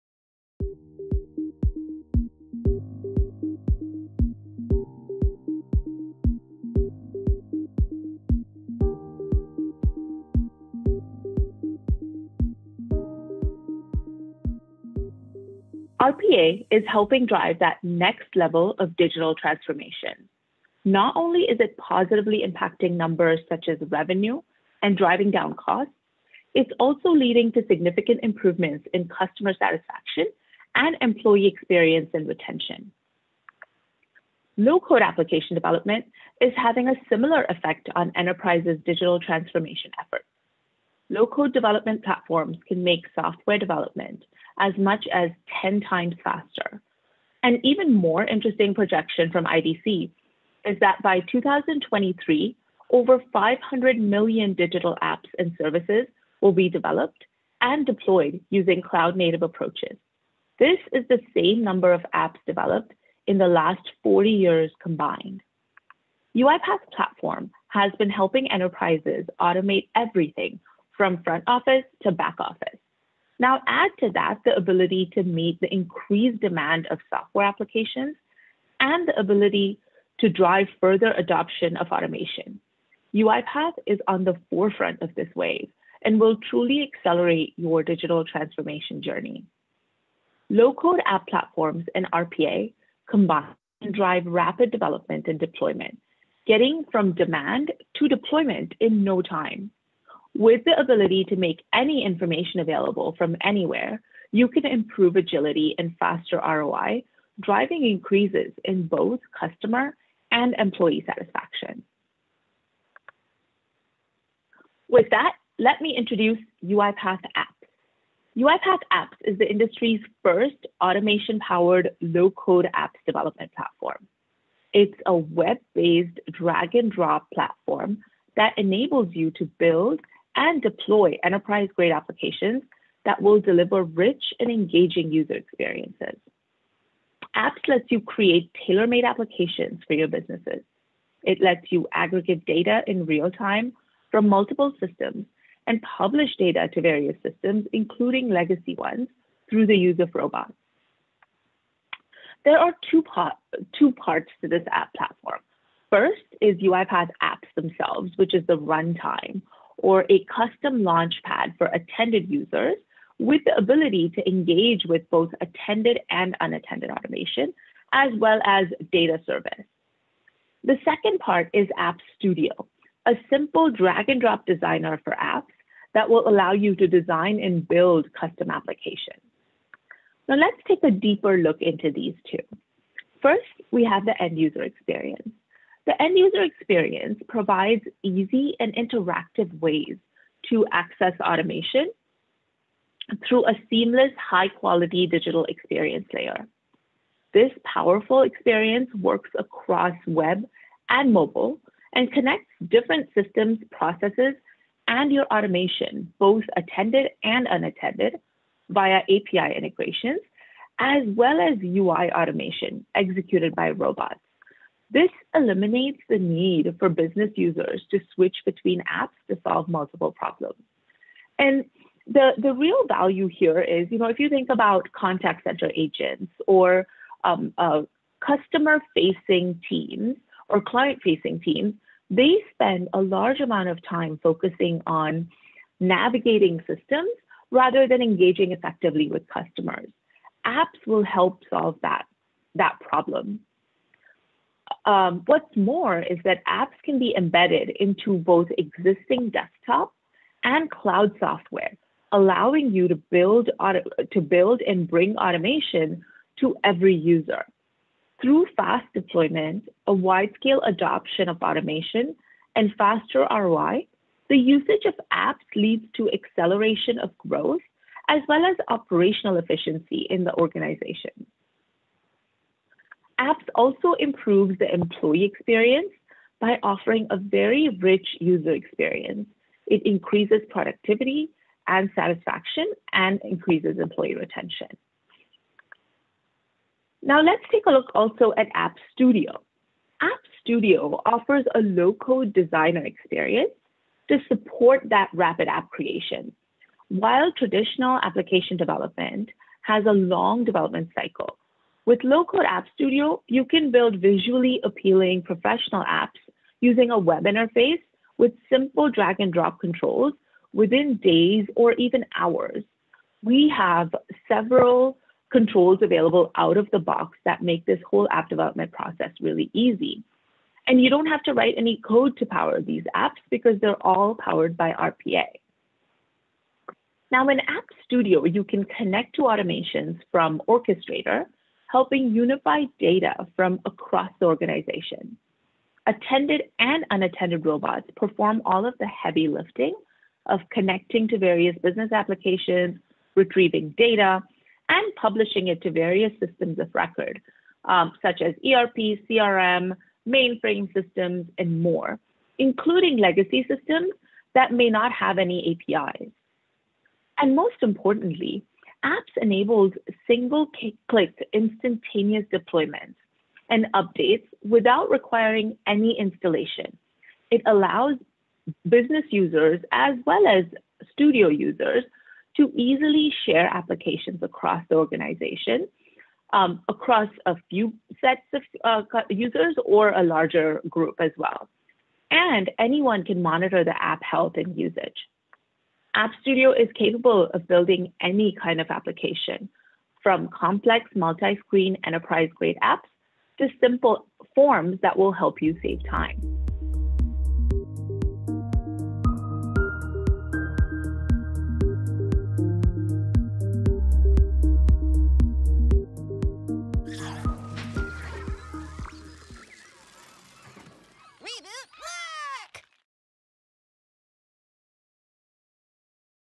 UiPath Apps 20.10 Overview - Low-Code Tool to Create Professional Applications
Explore how you can start building apps with no coding skills
How to connect all the different systems and create new useful dashboards, while the IT team is busy? Do it yourself! Explore how UiPath Apps can help you design professional-level applications that deliver rich and engaging end-user experiences.